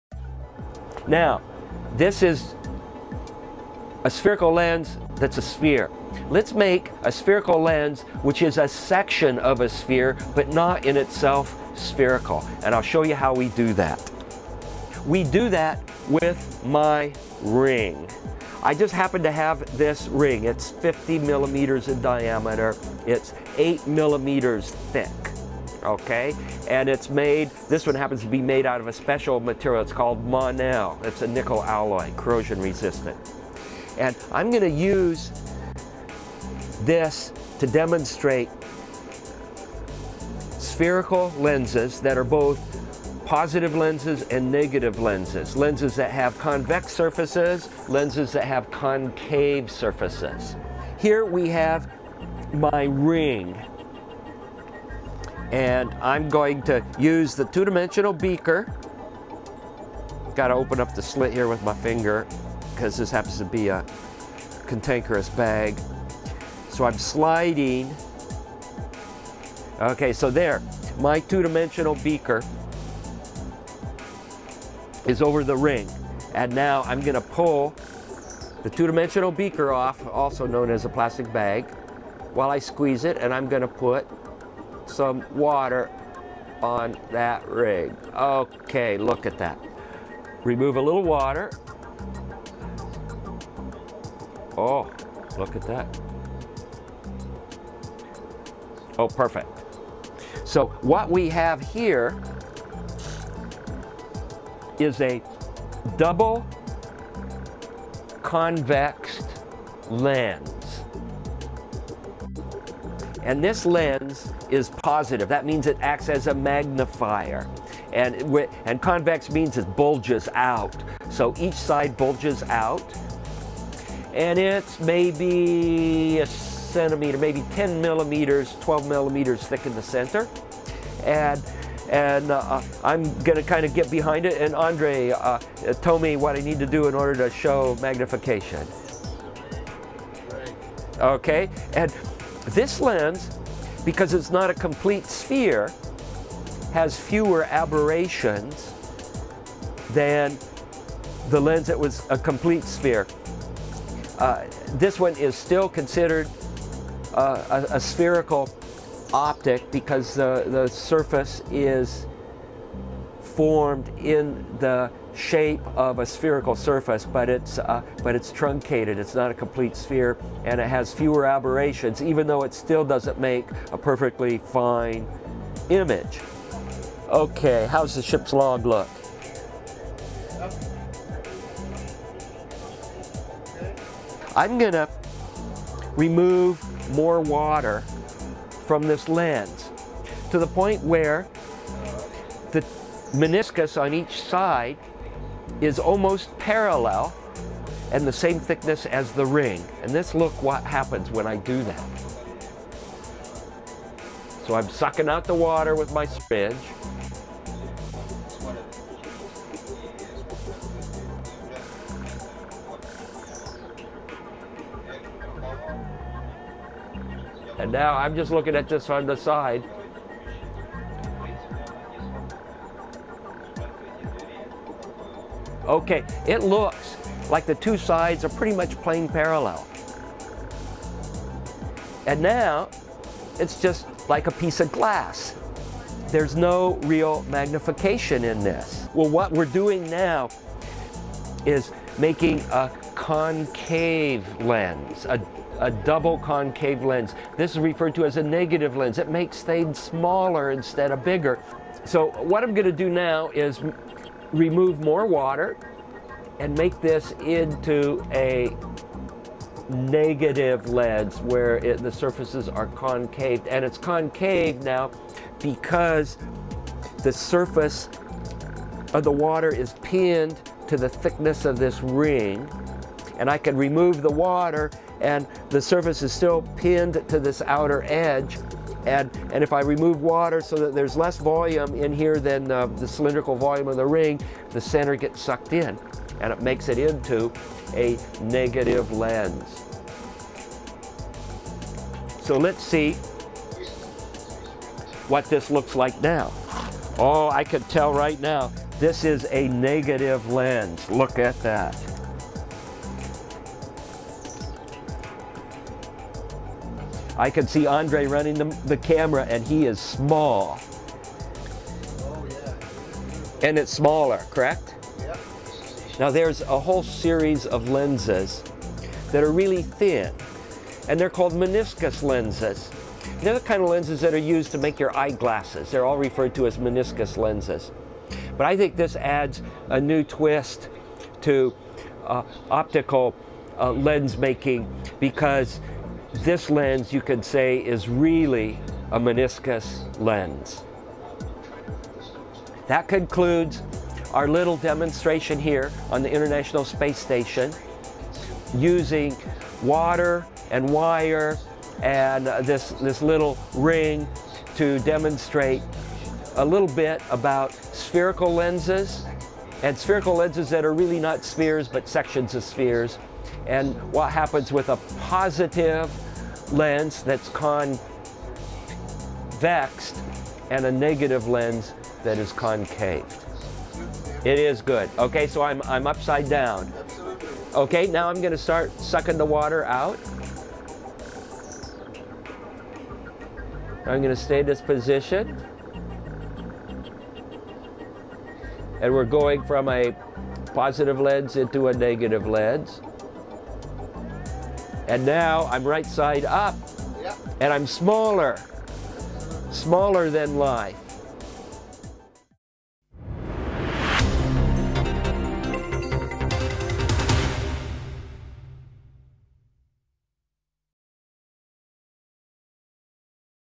Convex and Concave Water Lenses
While on the space station, astronaut Don Pettit demonstrates the optical properties of concave and convex lenses using a lens made of water.